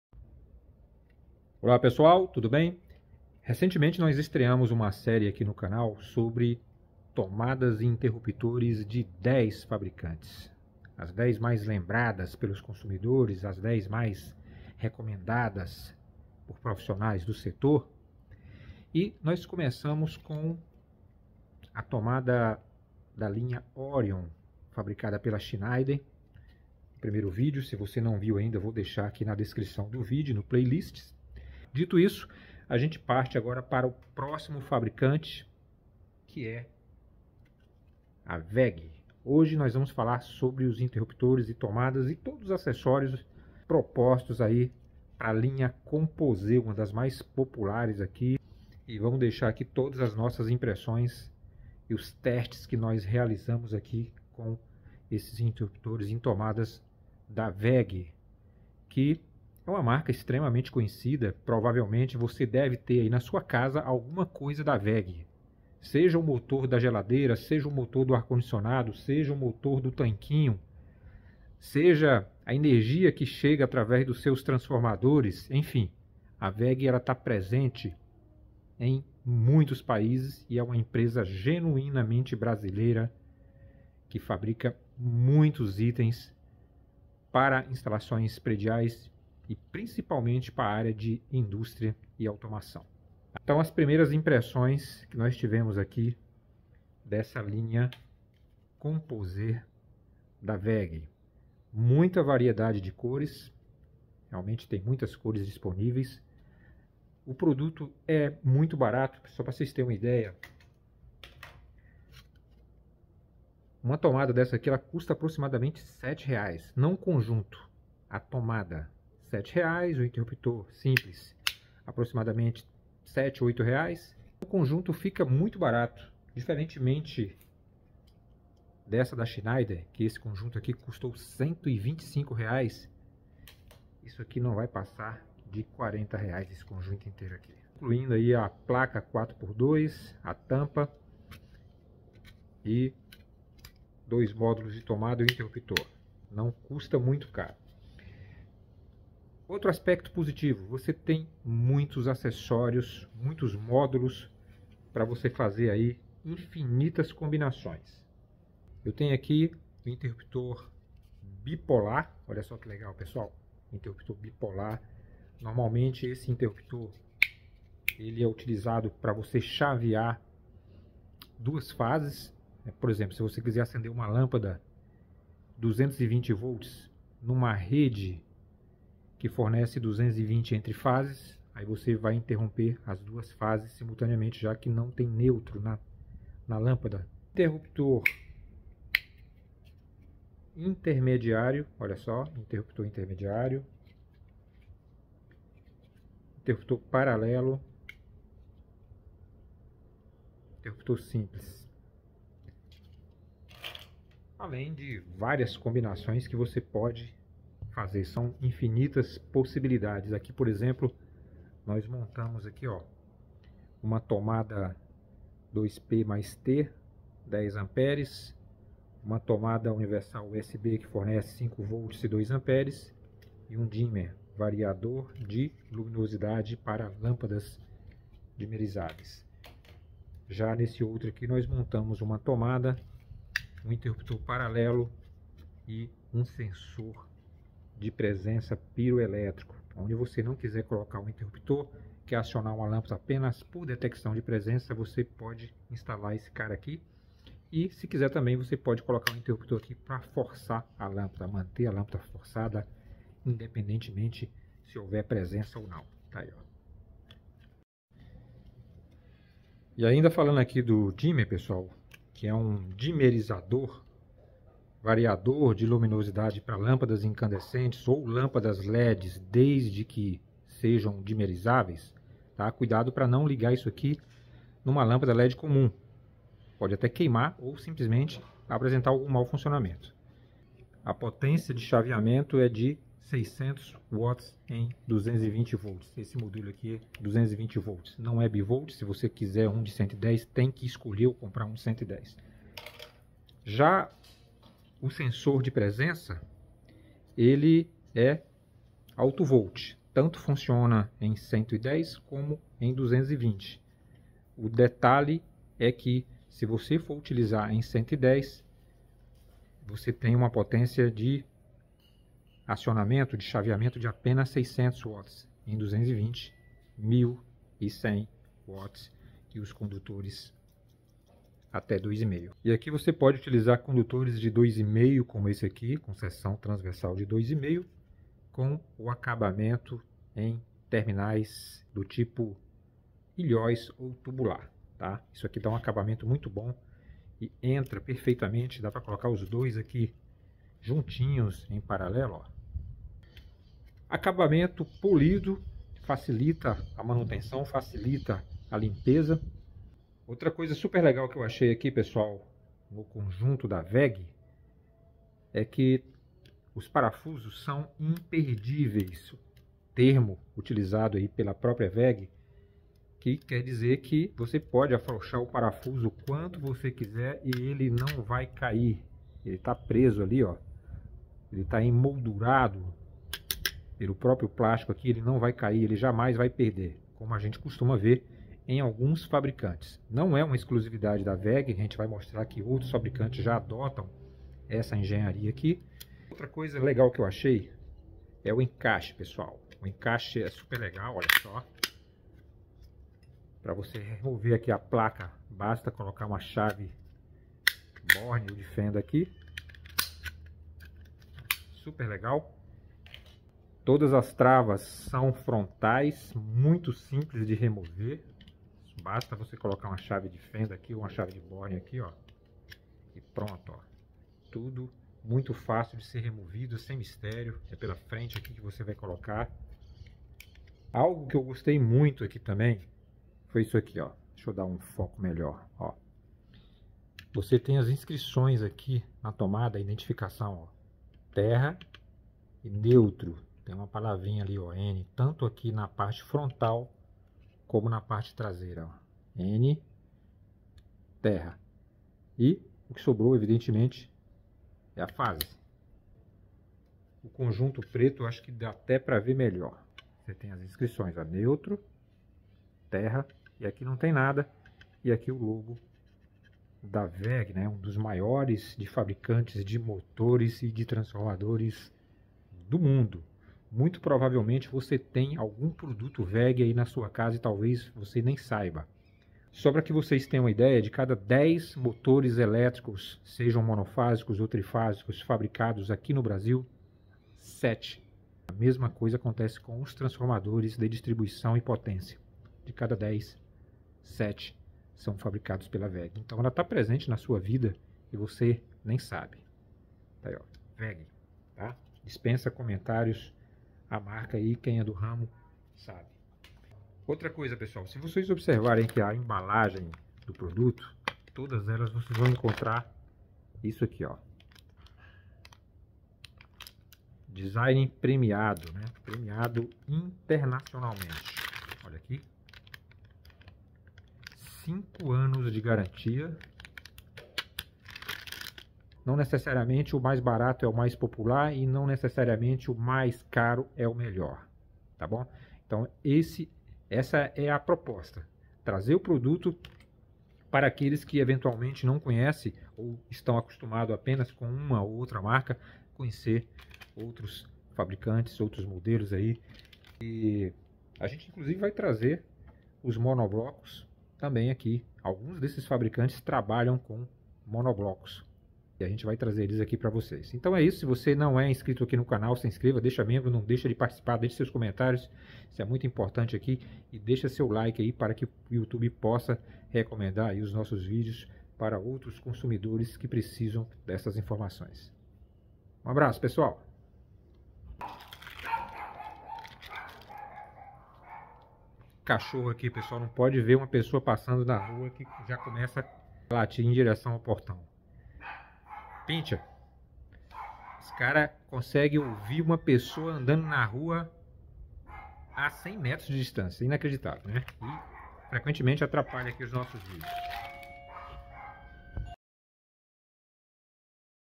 Hj vamos falar de tomadas e interruptores WEG - Compozé . Vc vai se surpreender c/ as possibilidades
Faça o curso de eletricista industrial 120h do Canal da Eletricidade , o mais vendido do Brasil .

A linha Composé é formada por suportes e placas que permitem montar 24 opções diferentes, ideais para todos os estilos e projetos residenciais ou empresariais. Para montar é fácil: é só unir o suporte aos módulos desejados e a placa de sua preferência. Simples, não?

São 10 opções de cores

Design moderno

Parafusos não ficam aparentes e não se perdem nos terminais dos módulos

Superfície polida de fácil limpeza e manutenção

Proteção antimicrobiana

Fácil instalação e retirada dos módulos

Fixa em caixas de passagem e permite a fixação em parede drywall